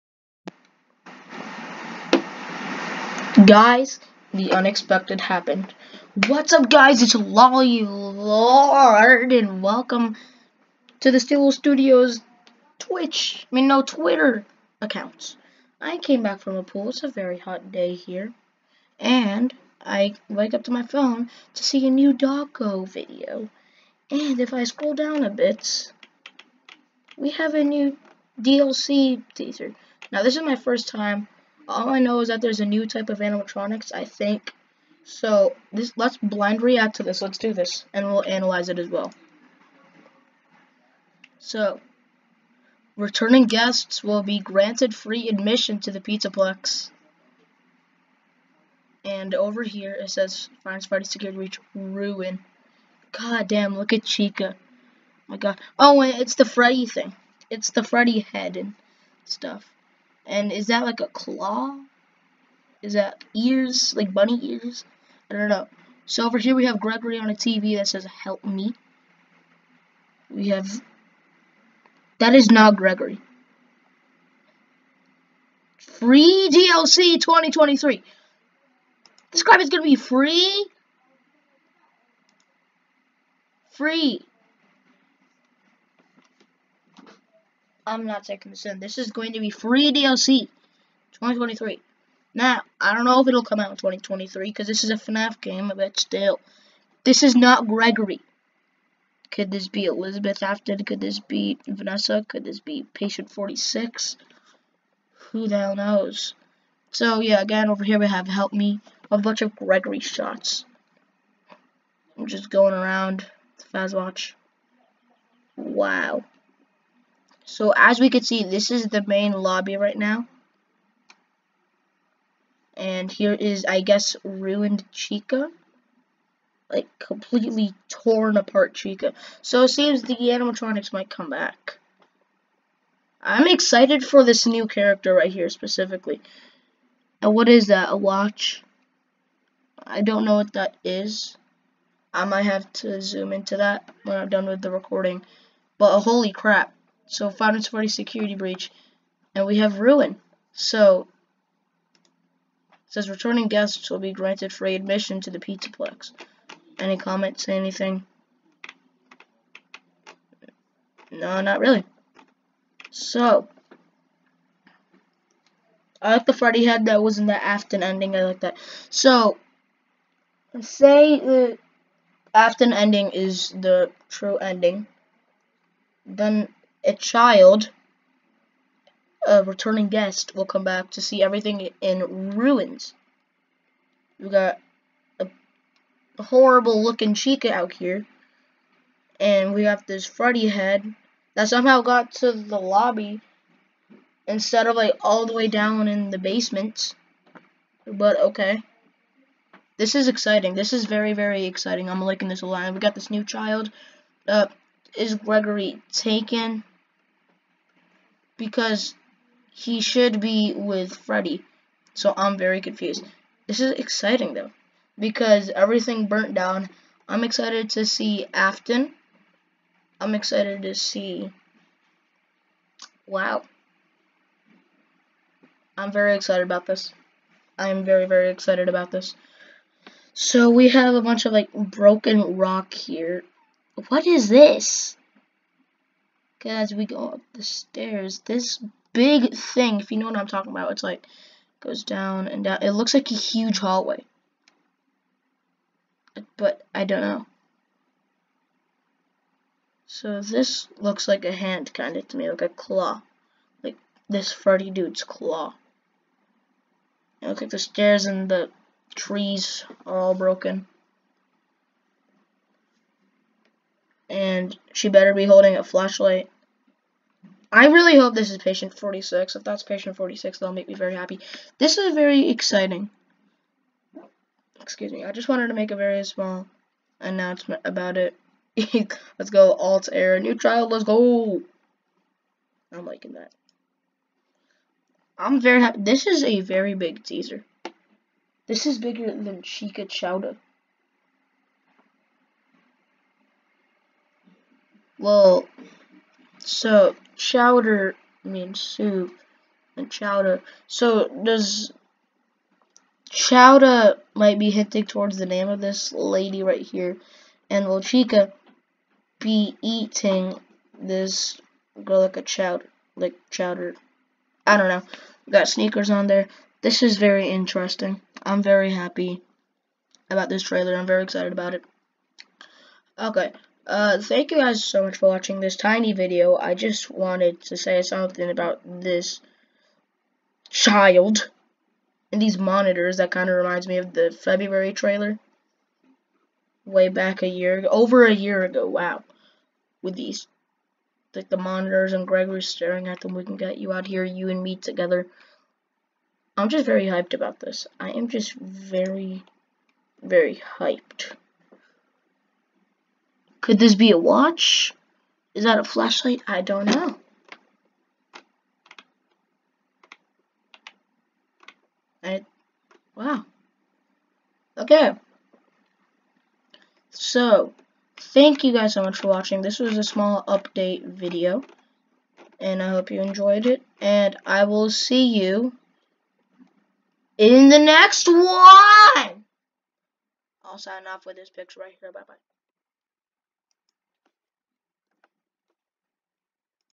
A NEW DLC TEASER CAME OUT & IT REVEALED A BIT TOO MUCH... (NEW CHILD!)
A new teaser released on the official Steel Wool Studios twitter page and it revealed so much that I cant take it.